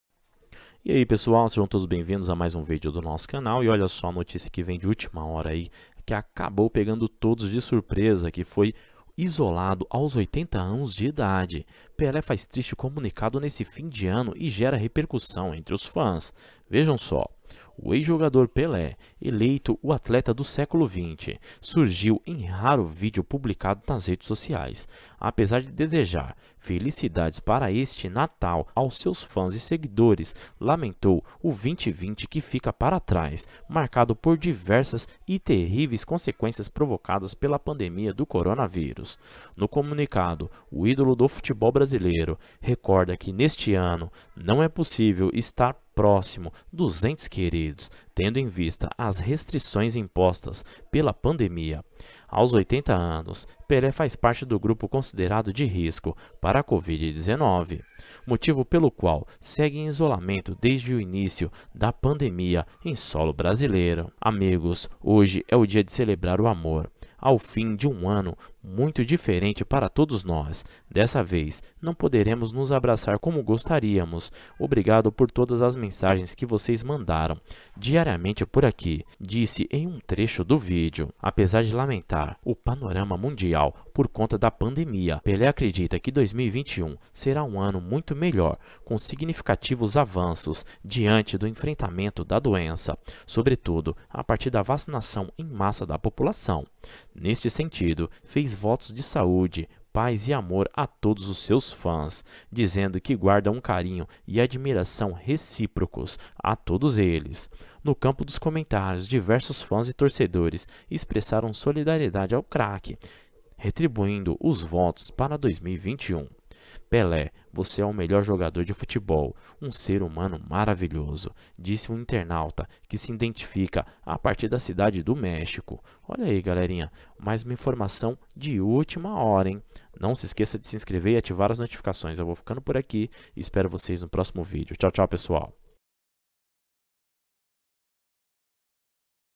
NA VIRADA DO ANO 2020/2021 DESCUBRA O QUE O REI PELÉ Isolado aos 80 anos, FALOU e faz triste comunic
DESCUBRA O QUE O REI PELE Isolado aos 80 anos, FALOU e faz triste comunicado neste fim de ano e gera. GALERINHA AJUDEM O CANAL DO NOSSO AMIGO CRESCER. INSCREVAM-SE